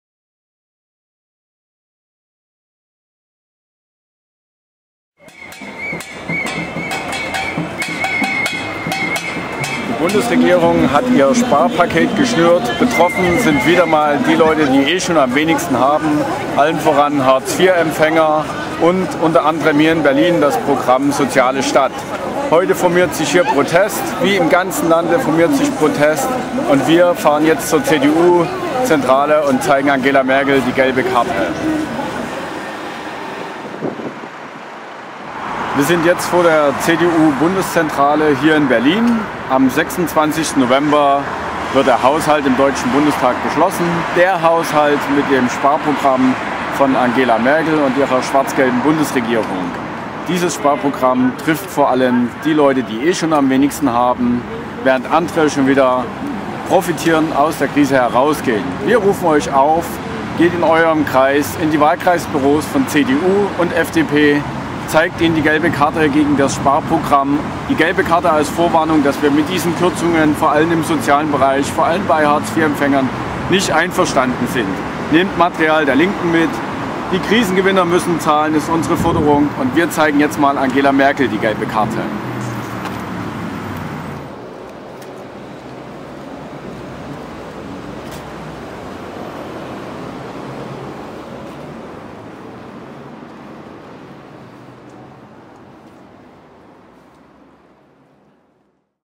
Zeigt die gelbe Karte
Die Bundesregierung will im Bundestag ein Sparpaket beschließen lassen, das die Reichen schont und die Armen noch ärmer macht. Geht zu den Bürgerbüros von Unions- und FDP-Abgeordneten und zeigt ihnen die gelbe Karte für ihre Sparpolitik.